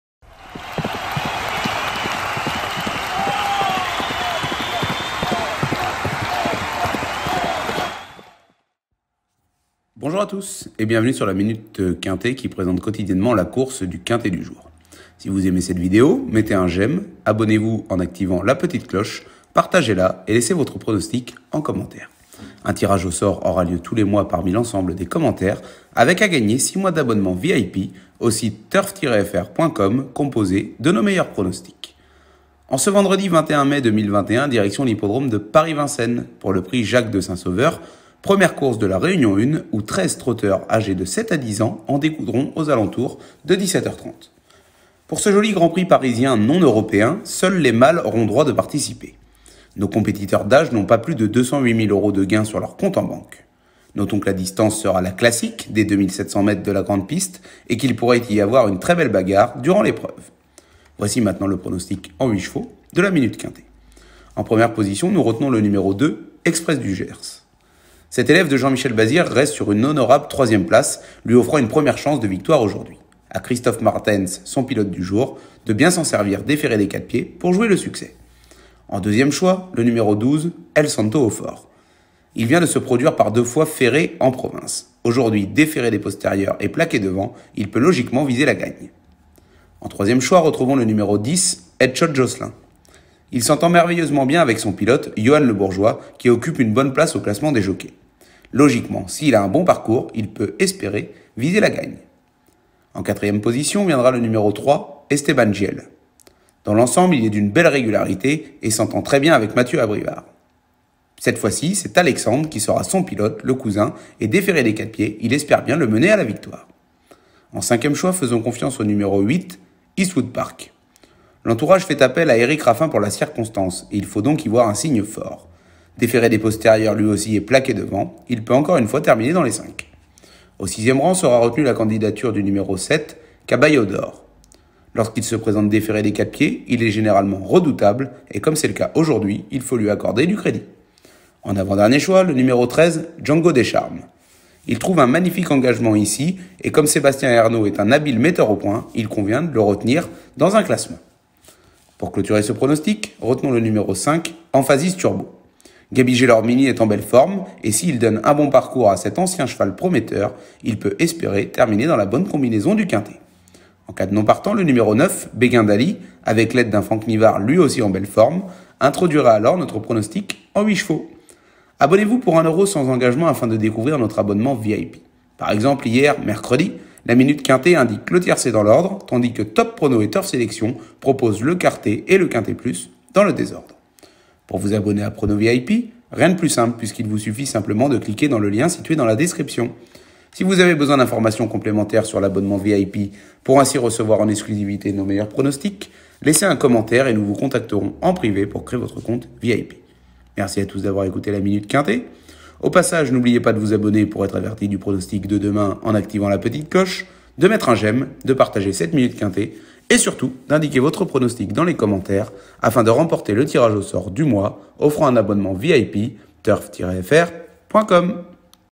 Minute Quinté TURF FR : PRIX JACQUES DE SAINT SAUVEUR - Vendredi 21 Mai Paris Vincennes PMU #240265
🥇 Retrouvez le pronostic exclusif de notre expert Turf Lucius Simenon pour le PRIX JACQUES DE SAINT SAUVEUR le Vendredi 21 Mai 2021 à l'hippodrome de Paris Vincennes en France.

✅ NOS LOGICIELS TURF :
✔️ Logiciel TURF INFO Professionnel Edition: